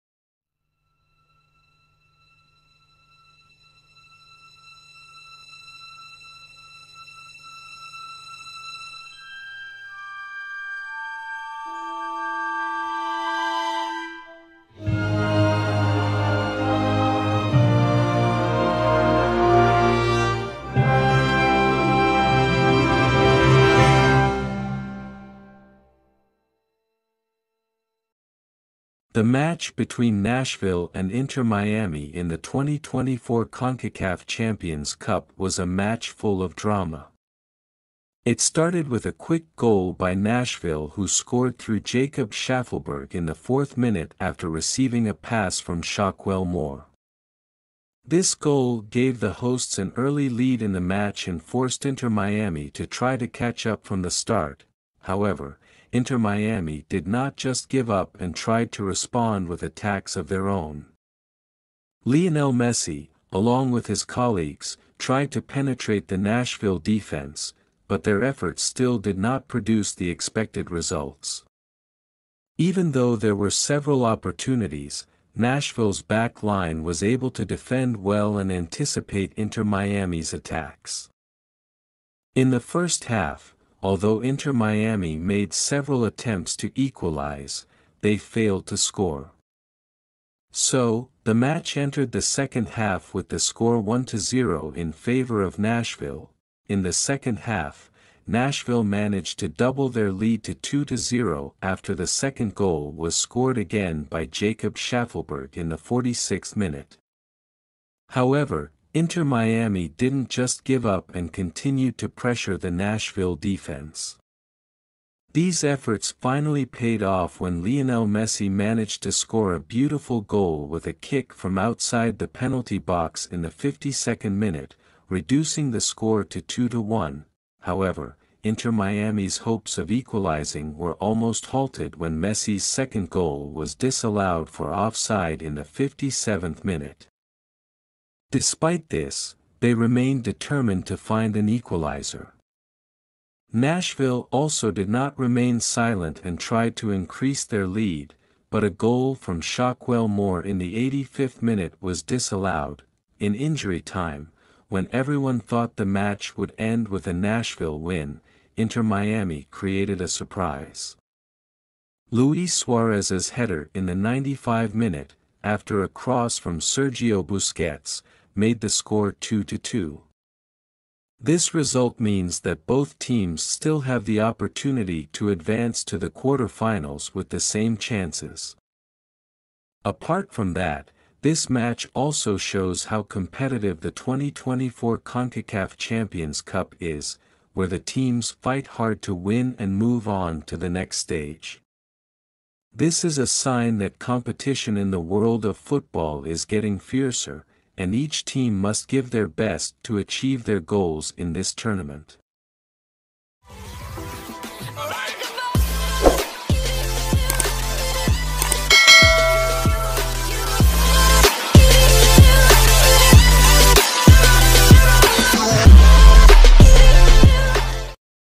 Nashville vs Inter Miami 2-2 | Match Highlights & All Goals | Lionel Messi & Luis Suarez Score Goals
The match between Nashville and Inter Miami in the 2024 Concacaf Champions Cup was a match full of drama. It started with a quick goal by Nashville who scored through Jacob Shaffelburg in the fourth minute after receiving a pass from Shaquell Moore. This goal gave the hosts an early lead in the match and forced Inter Miami to try to catch up from the start.

However, Inter Miami did not just give up and tried to respond with attacks of their own. Lionel Messi, along with his colleagues, tried to penetrate the Nashville defense, but their efforts still did not produce the expected results. Even though there were several opportunities, Nashville's back line was able to defend well and anticipate Inter Miami's attacks.

In the first half, although Inter Miami made several attempts to equalize, they failed to score. So, the match entered the second half with the score 1-0 in favor of Nashville.

In the second half, Nashville managed to double their lead to 2-0 after the second goal was scored again by Jacob Shaffelburg in the 46th minute. However, Inter Miami didn't just give up and continued to pressure the Nashville defense. These efforts finally paid off when Lionel Messi managed to score a beautiful goal with a kick from outside the penalty box in the 52nd minute, reducing the score to 2-1.

However, Inter Miami's hopes of equalizing were almost halted when Messi's second goal was disallowed for offside in the 57th minute. Despite this, they remained determined to find an equalizer. Nashville also did not remain silent and tried to increase their lead, but a goal from Shaquell Moore in the 85th minute was disallowed.

In injury time, when everyone thought the match would end with a Nashville win, Inter Miami created a surprise. Luis Suarez's header in the 90+5 minute, after a cross from Sergio Busquets, made the score 2-2. This result means that both teams still have the opportunity to advance to the quarter-finals with the same chances.

Apart from that, this match also shows how competitive the 2024 Concacaf Champions Cup is, where the teams fight hard to win and move on to the next stage. This is a sign that competition in the world of football is getting fiercer, and each team must give their best to achieve their goals in this tournament.

inter miami vs nashville leagues cup final full match,